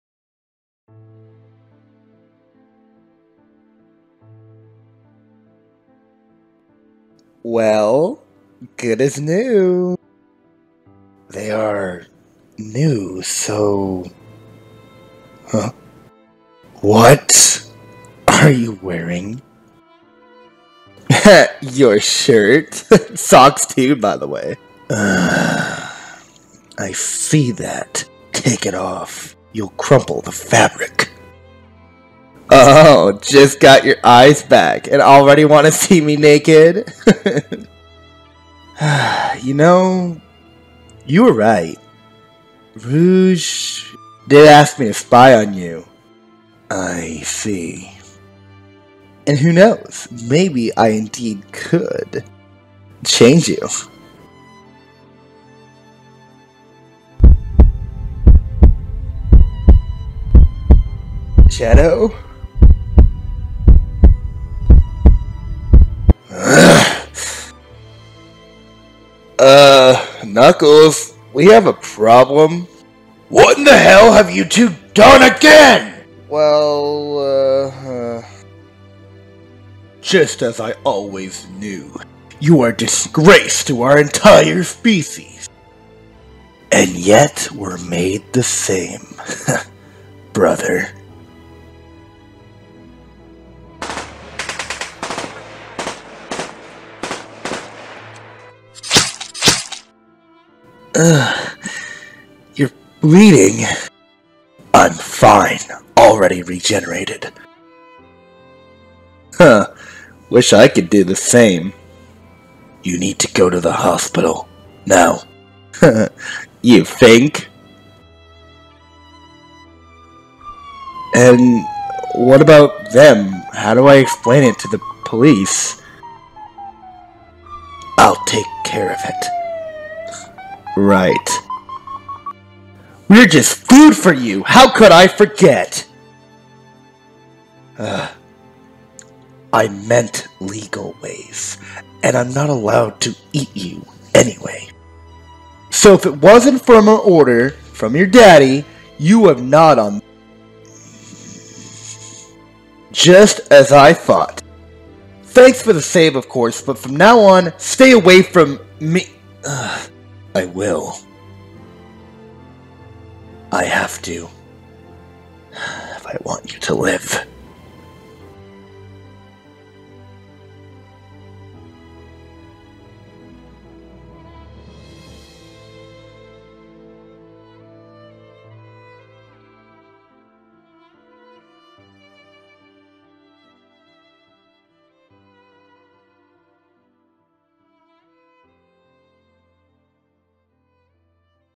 Sonic Comic Dub - White Collars - Part 2
Welcome to the very hyped second part of "White Collars" this will be the last of the white collars as I am waiting for more panels/artwork of the story! So this will be on hold until I get enough for a part 3

I still hope as always you enjoy the video!

All music and comic/artwork goes to their owners (info linked below)

VA is all done by me. Hoping to have a bunch of more people join someday!

Thank you so much for reading and watching :)